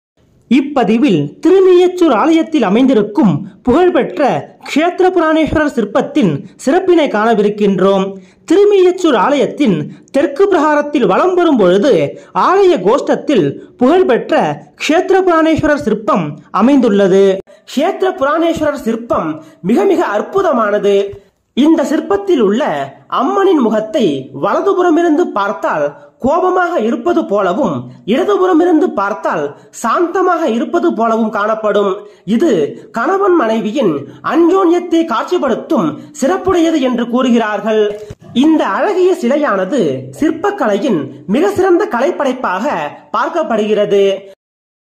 திருமீயச்சூர் சேத்திர புராணேஸ்வரர் சிற்பத்தின் சிறப்புகள் thirumeeyachur Kshetra puraneswarar
The sculpture of Kshetra Puraneswarar is very magnificent.  The face of the goddess in this is seen to be angry when viewed from the right and calm when viewed from the left.  It is said that it is special to show the intimacy of husband and wife.  this beautiful sculpture

 Sun God stayed at Tirumeyachur to get rid of the curse of Shiva.  The voice of the sun crying out for quick deliverance from the severity of the curse affected the lonely Ambal.  So she gets angry and turns to curse the sun.  This world has been greatly affected by the curse I have already given by holding Swami Ambal's muzzle.  He said don't curse.

This view is designed as Chetra Puraneswarar sculpture.  If you look at this sculpture from the right side, Ambal is very angry and from the left side, you can see that he is meek. This statue is a masterpiece of sculpture.

க்ஷேத்திர புராணேஸ்வரர் சிற்பம் மிகவும் அற்புதமானது. இதிலுள்ள அம்மனின் முகத்தை வலதுபுறமிருந்து பார்த்தால் கோபமாக இருப்பது போலவும், இடதுபுறமிருந்து பார்த்தால் சாந்தமாக இருப்பதை போலவும் தெரியும். இது கணவன் மனைவியின் அன்யோன்யத்தை காட்சிபடுத்தும் சிறப்புடையது என்று கூறுகிறார்கள்.

 இந்த அழகிய சிற்பம் சிற்பக்கலையின் உச்சம் என்றாலும் மிகையாகாது.

 சிவசாபத்திலிருந்து விமோசனம் பெற சூரிய தேவன் திருமீயச்சூரில் தங்கினான். சாபத்தின் கடுமையால் சீக்கிரம் விமோசனம் வேண்டி சூரியன் அலறிய குரல், ஏகாந்தமாய் இருந்த அம்பாளுக்கு பாதிப்பை உண்டாக்கியது. அதனால் கோபமடைந்து, சூரியனை சபிக்கத் திருப்புகிறாள். சுவாமி அம்பாளின் முகவாயைப் பிடித்து நான் ஏற்கனவே கொடுத்த சாபத்தினால் இவ்வுலகம் மிகவும் பாதிக்கப்பட்டு விட்டது. நீயும் சபிக்காதே என்றார்.

 இந்தக் காட்சி சேத்திர புராணேஸ்வரர் சிற்பமாக வடிவமைக்கப்பட்டுள்ளது. இந்த சிற்பத்தை வலது புறமாய் இருந்து பார்த்தால் அம்பாள் மிக கோபமாகவும், இடது புறமாய் இருந்து பார்த்தால் சாந்தமாகவம் தெரியும்.
 இந்த சிலையானது சிற்பக் கலையின் மிகச்சிறந்த கலைப்படைப்பாகும்.

Bharatha Thamizhan
Abishek Indradevan
அபிஷேக் இந்திரதேவன்
பாரத தமிழன்